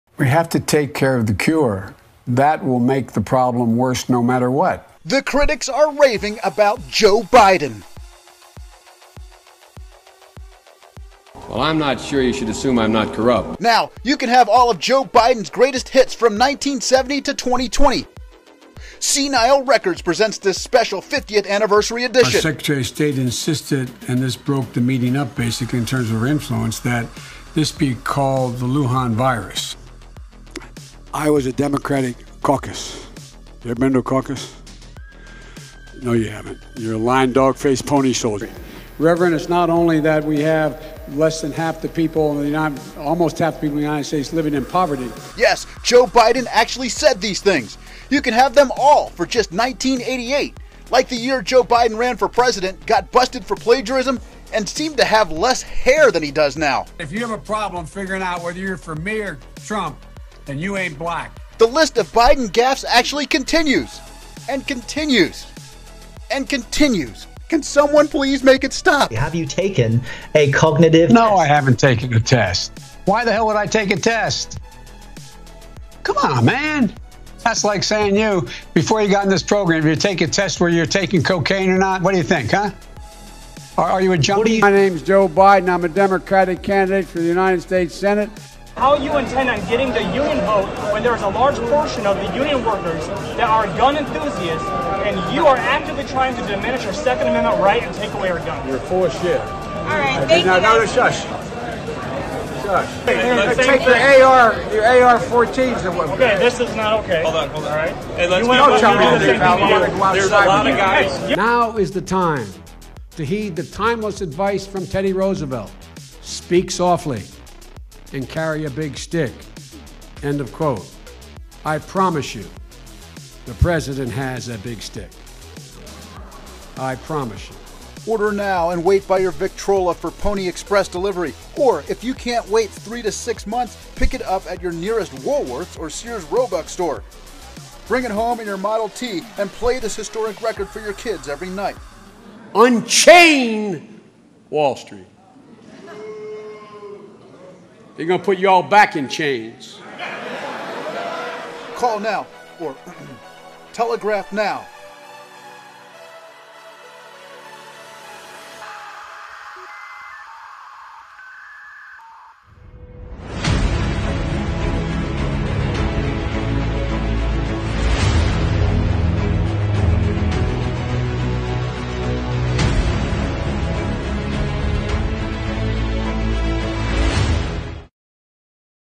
Joe Biden's Greatest Hits! Available for Pony Express Delivery!
I am NOT a Trump supporter by any means, but I mean come on. How was this man EVER an option for a possible Leader of a country??? This video speaks for itself, Joe Biden should have been Committed years ago...
Mirrored from Americans for Limited Government